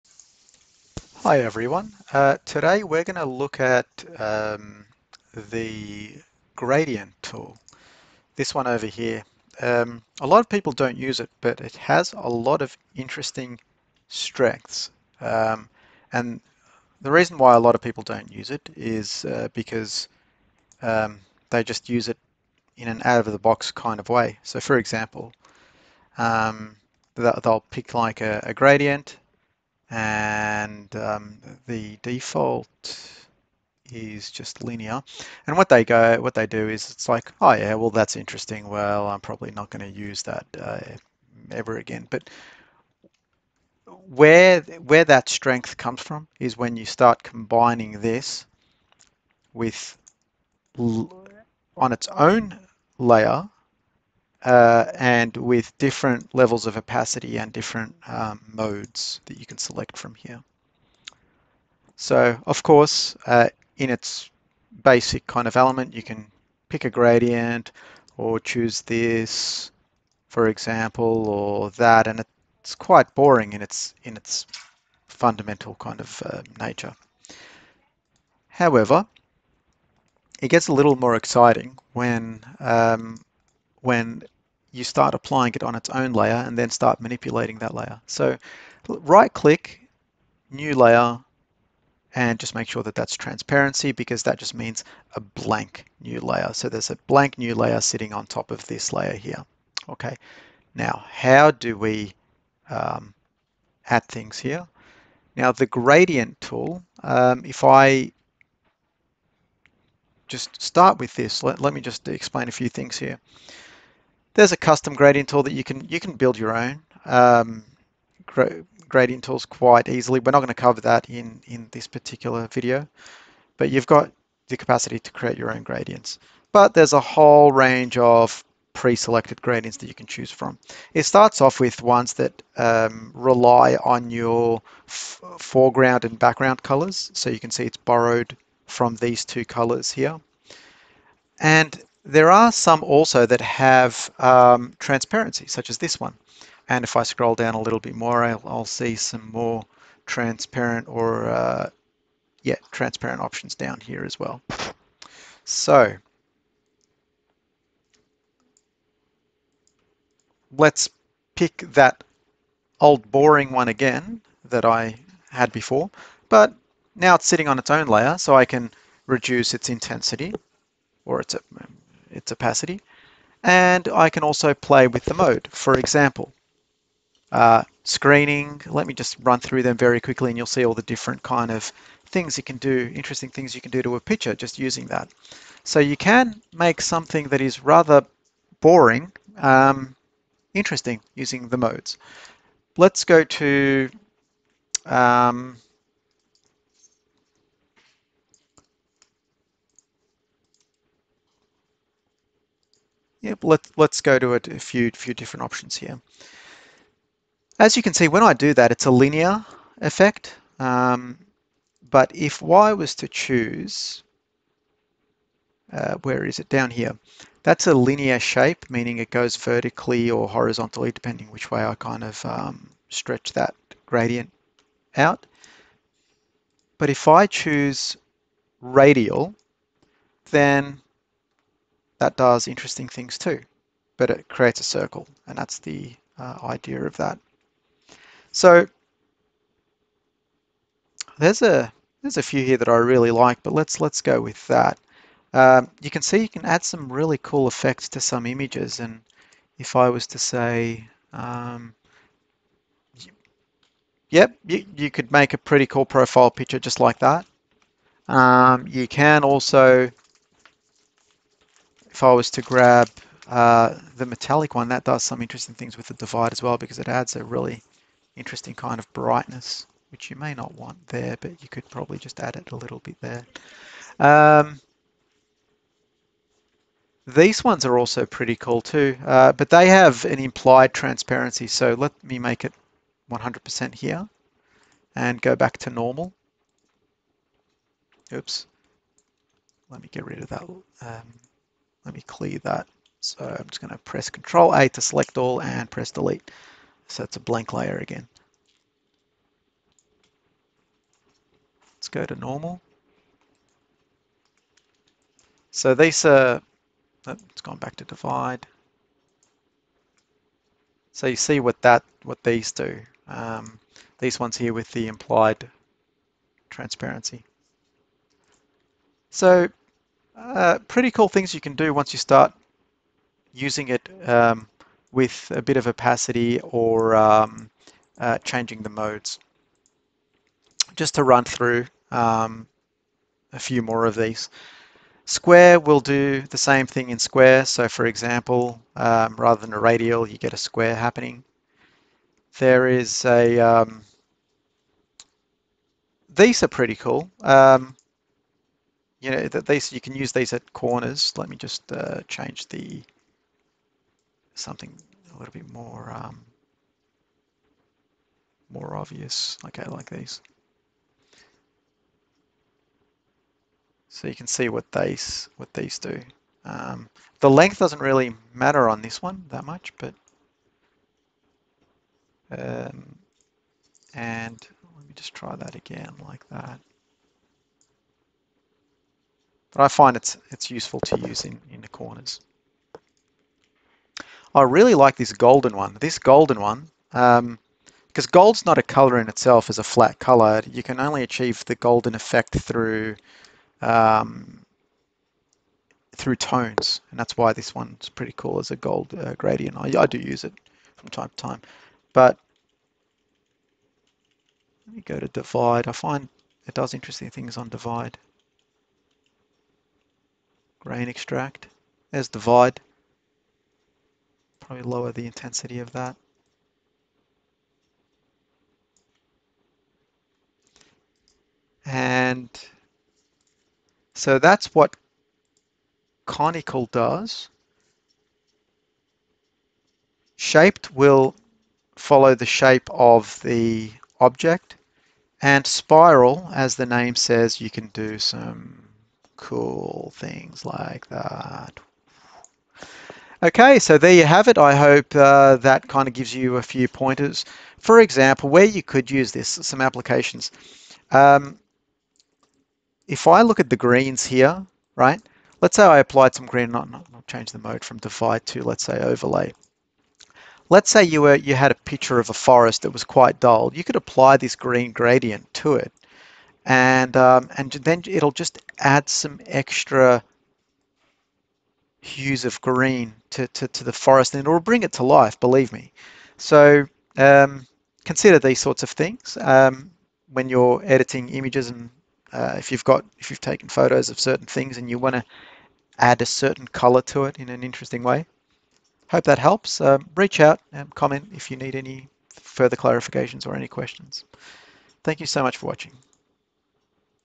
GIMP Tutorial: Gradient Tool - Making the most of it!
One of the lesser used tools in GIMP, but it has some good applications!  Try it out!